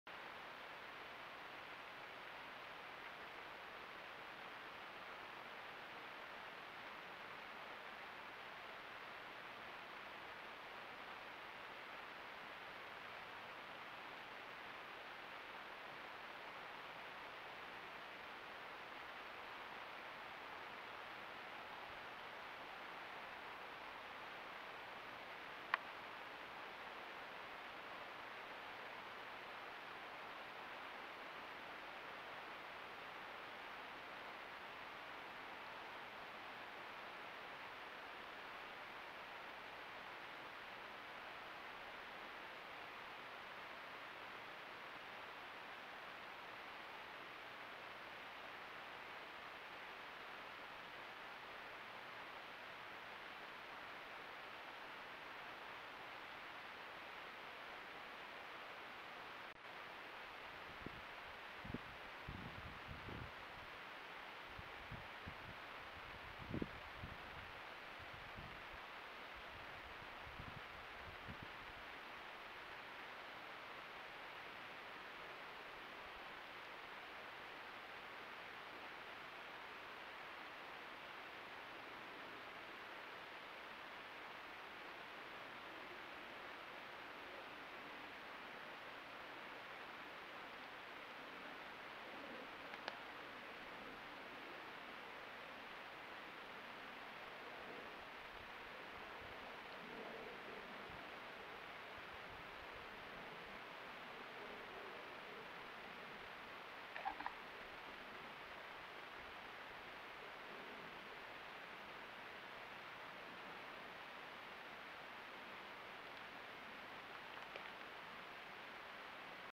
Bull Moose Footage - Wyoming Trail Cam
A trail camera in Wyoming's Snowy Range captured footage of this bull moose early in the summer of 2021.